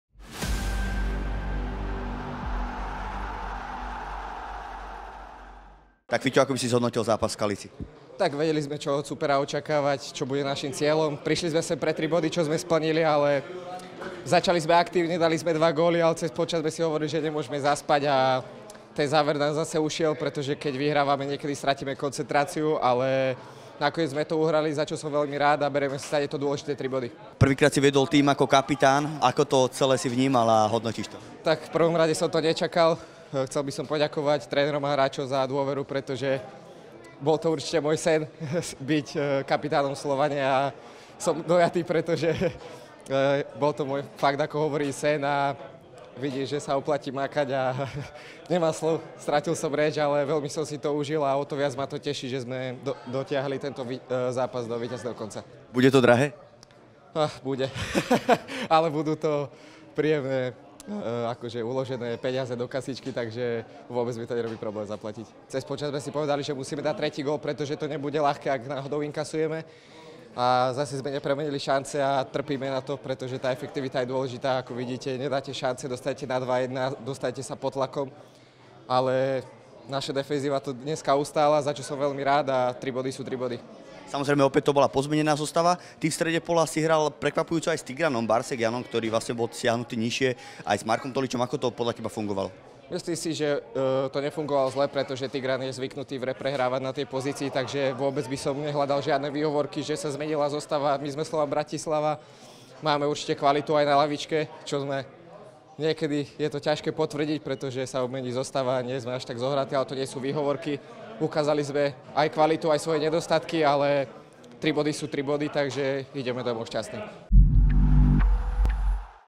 REVIEW | MFK Skalica - ŠK Slovan Bratislava | 1:2 | Filip LICHÝ
Dojatý Fiťo: "Byť kapitánom Slovana bol vždy môj sen."